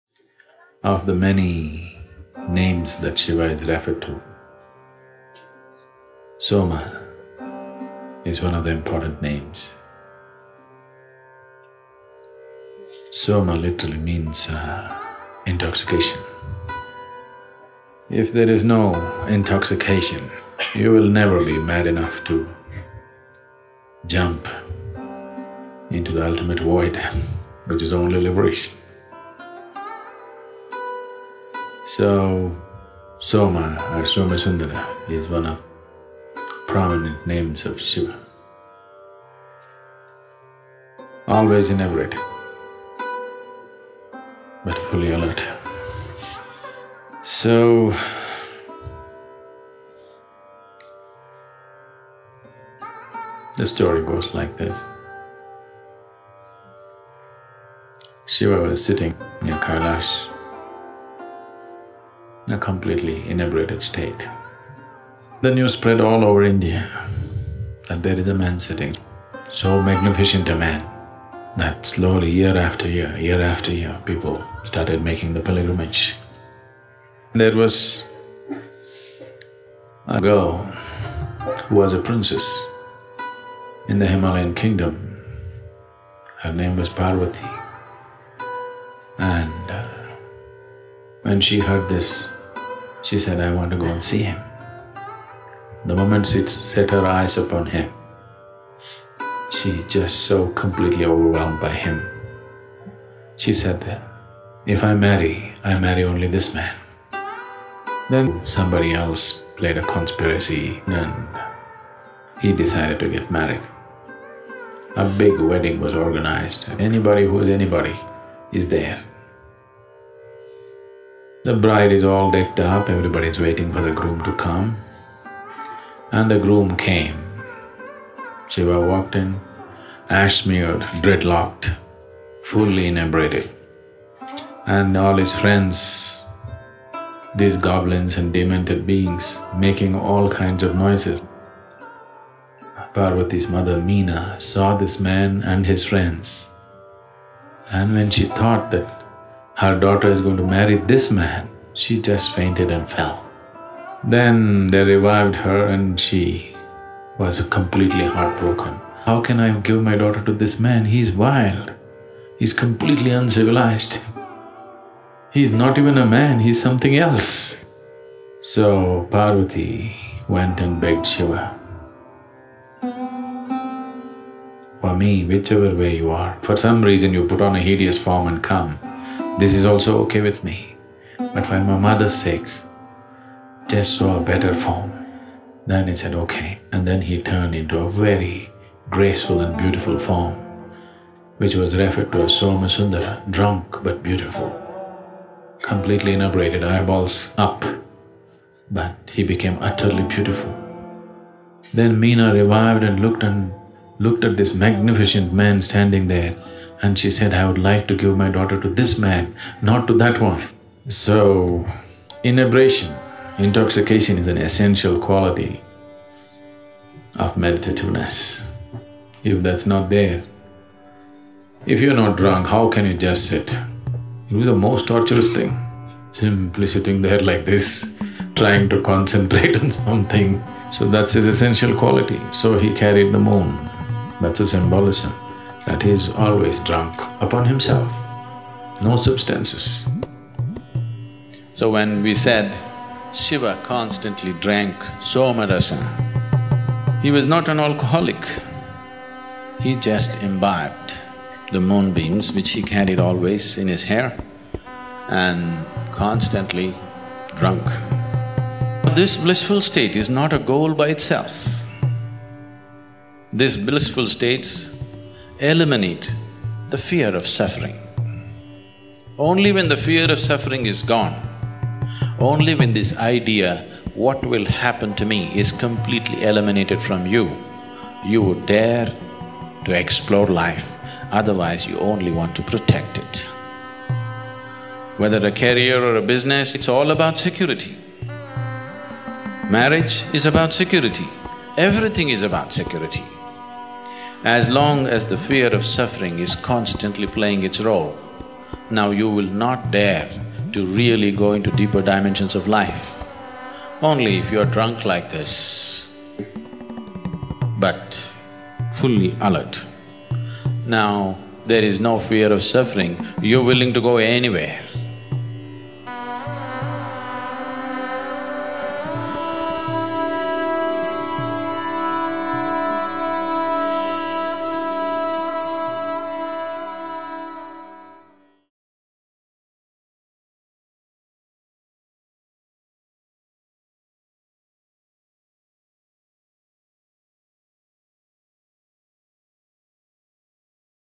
Why & How Shiva Is Always Intoxicated
Sadhguru tells the story of the Somasundara form of Shiva, and how Shiva is always inebriated but fully alert.